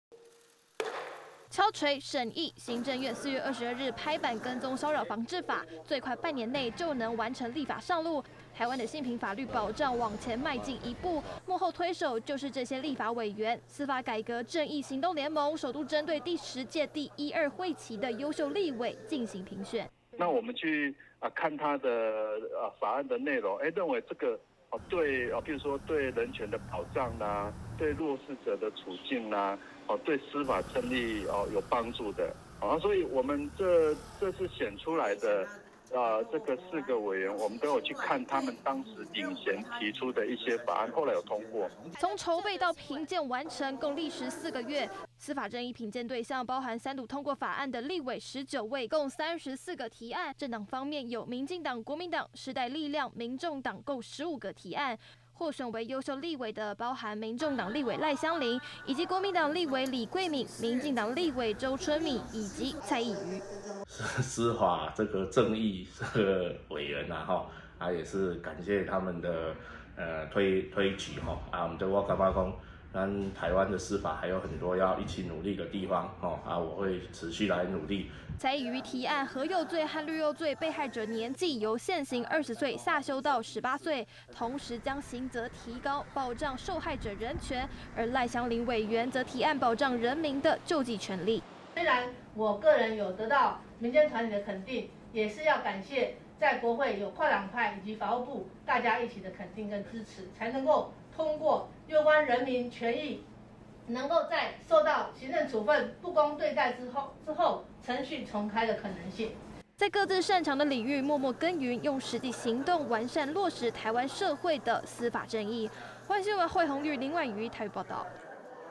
推動跟騷法 守護人權 民團評鑑4優秀立委｜寰宇新聞20210603
立法院本會期剛結束，司改正義行動聯盟公布，4位表現優秀的委員，點名包括民眾黨賴香伶、國民黨李貴敏和民進黨的周春米及蔡易餘，他們積極推動法案，讓人權保障、弱勢者處境關懷等，修法完善制度，取得進展。